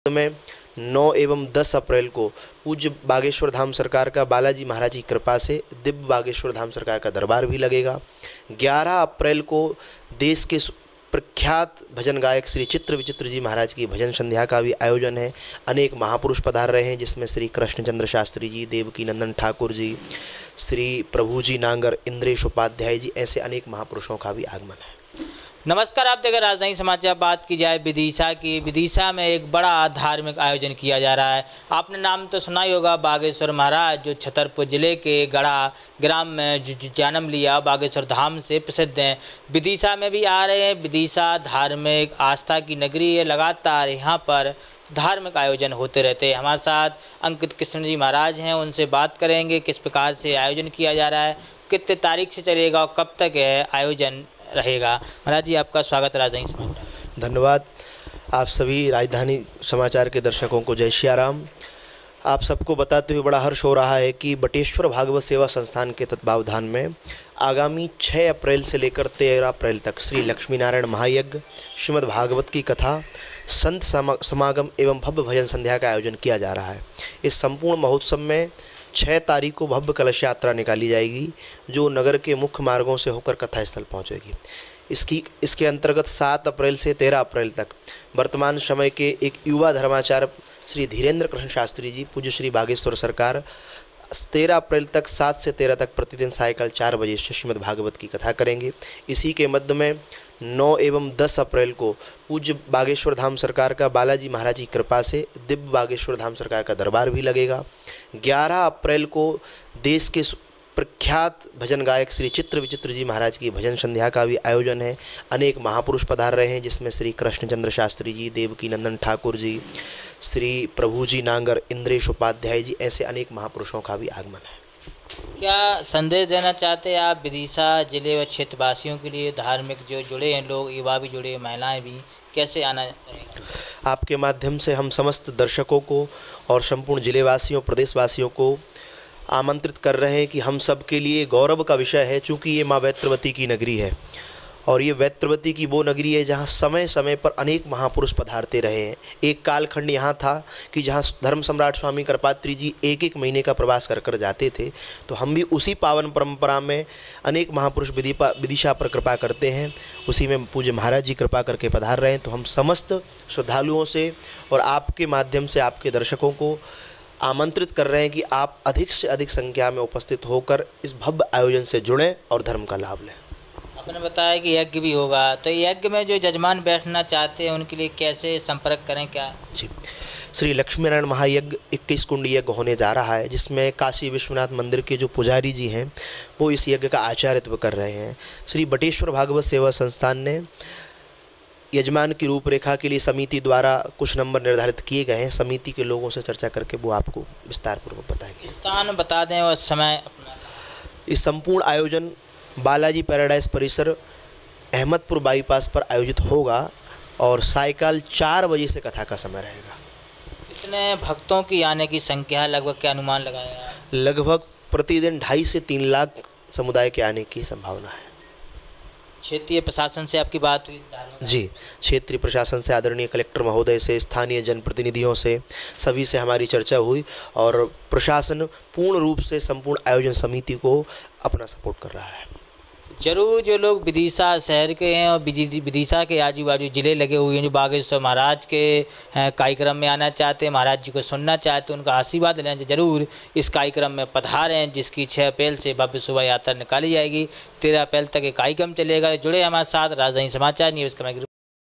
विदिशा में लगेगा बागेश्वर धाम सरकार का दिव्य दरबार 9 और 10 अप्रैल को।, पंडित अंकितकृष्ण वटुक जी
विदिशा में लगेगा बागेश्वर धाम सरकार का दिव्य दरबार 9 और 10 अप्रैल को..।